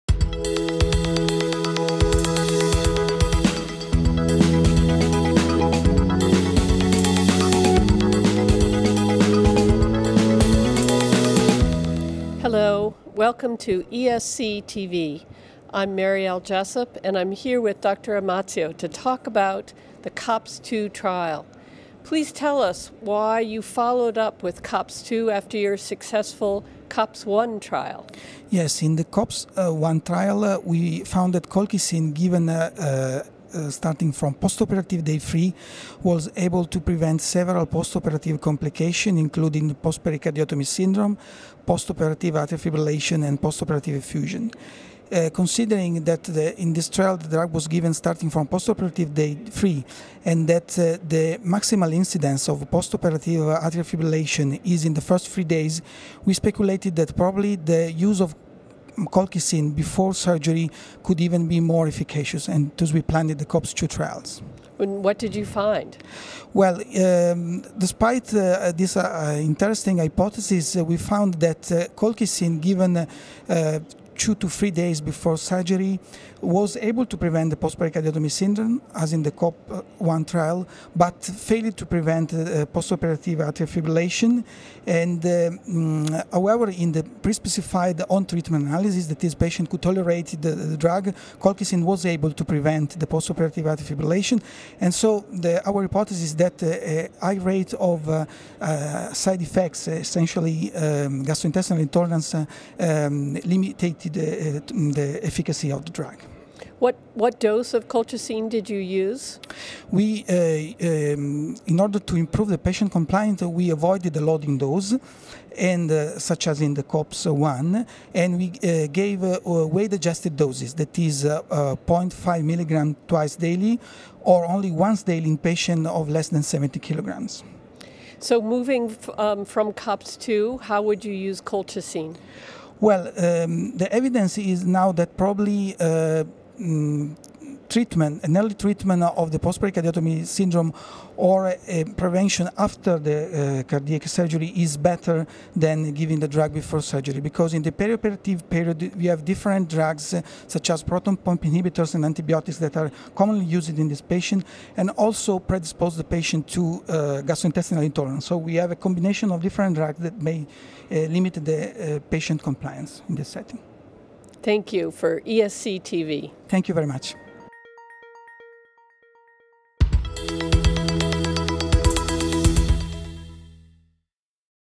ESC TV 2014 - Colchincine effective against some post-op events
HOTLINE I : CoIchincine for Prevention of the Post-pericardiotomy Syndrome and Post-operative Atrial Fibrilation (COPPS-2 trial)

Interviewee : Dr.  Massimo Imazio, FESC, Torino, Italy
Interviewer : Prof. Mariell Jessup, FESC, Philadelphia, United States of America